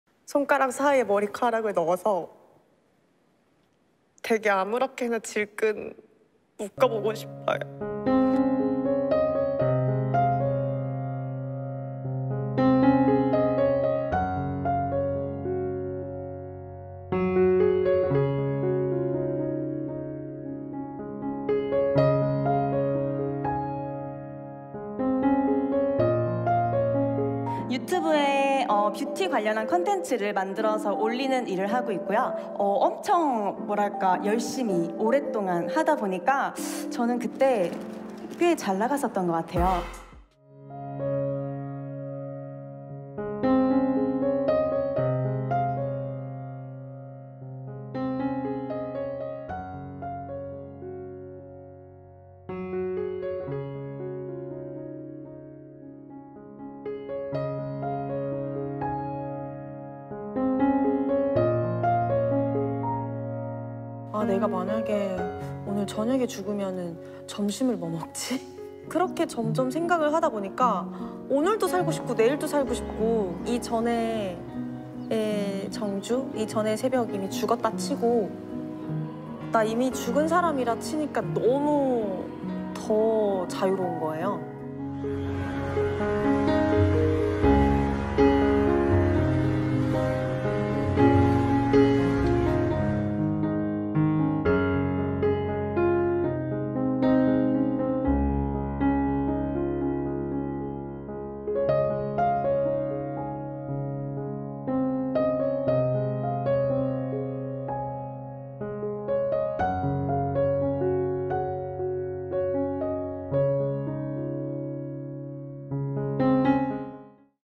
유튜버 새벽, 생전 털어놨던 속마음···쏟아지는 추모 물결
(서울=뉴스1) 박혜성 기자 = 유튜버 새벽(본명 이정주·30)이 혈액암 투병 끝에 세상을 떠났다.

새벽의 소속사인 아이스크리에이티브 김은하 대표는 지난 30일 자신의 인스타그램에 "빛나고 소중했던 나의 오랜 친구이자 소속 크리에이터 새벽, 이정주님이 오늘 아침 별이 되었습니다"라는 글을 게시하며 새벽의 사망 소식을 전했다.

자세한 내용은 영상으로 확인해 보시죠.